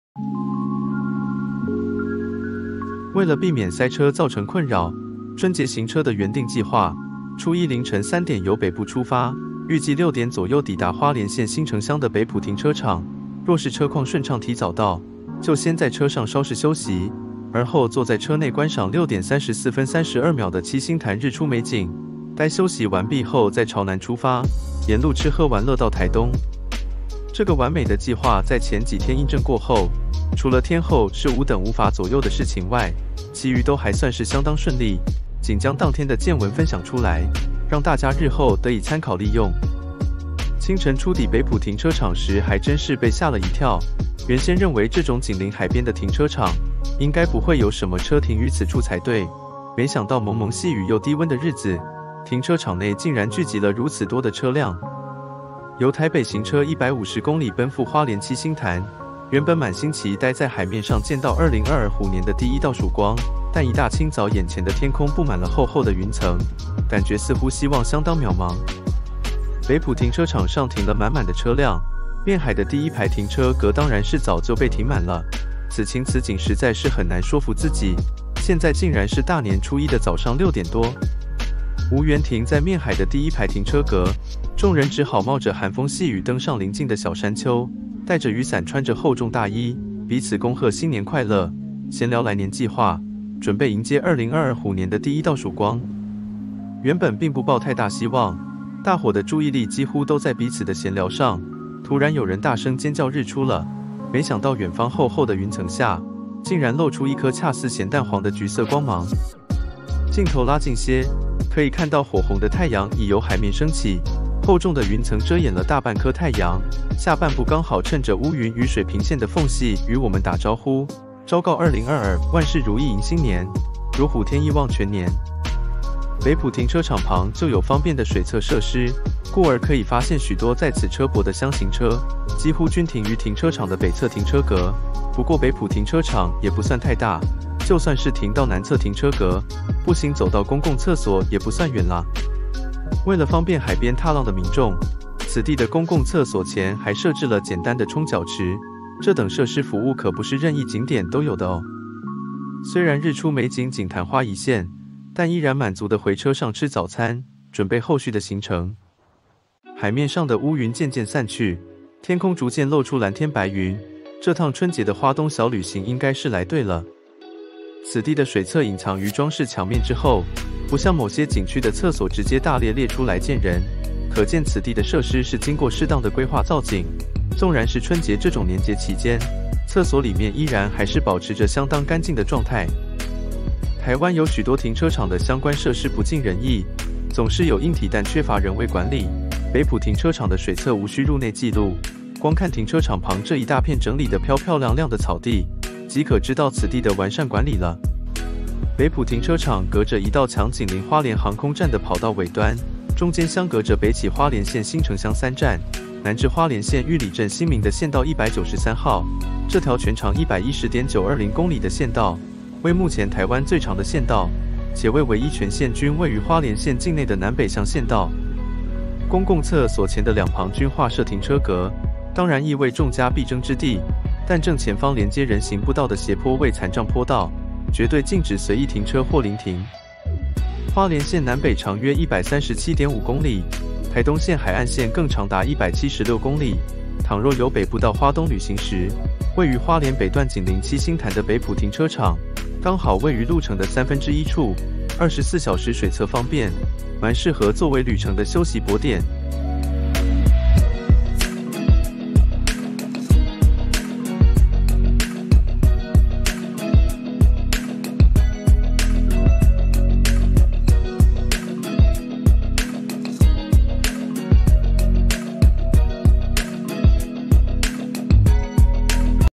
【車宿野營】張眼可見日出美景的車泊好所在 - 花蓮七星潭北浦停車場 - 阿舍的精彩生活
為了避免塞車造成困擾，春節行車的原定計畫，初一凌晨三點由北部出發，預計六點左右抵達花蓮縣新城鄉的北浦停車場，若是車況順暢提早到，就先在車上稍事休息，而後坐在車內觀賞 06:34:32 的七星潭日出美景，待休息完畢後再朝南出發，沿路吃喝玩樂到台東。

這個完美的計畫在前幾天印證過後，除了天候是吾等人為無法左右的事情外，其餘都還算是相當順利，僅將當天的見聞分享出來，讓大家日後得以參考利用。